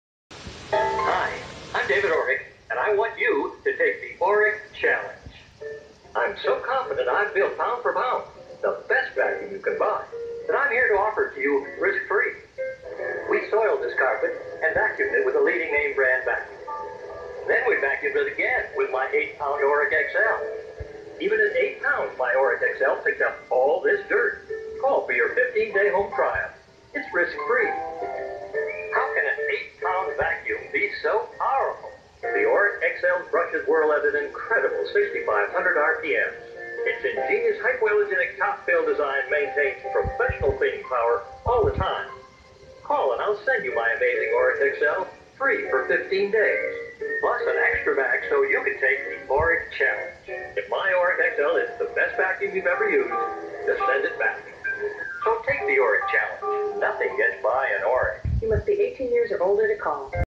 The Oreck Challenge (1998)
I find this commercial very fascinating. I mean, demonstrating on how to use a special vacuum such as Oreck is something to see.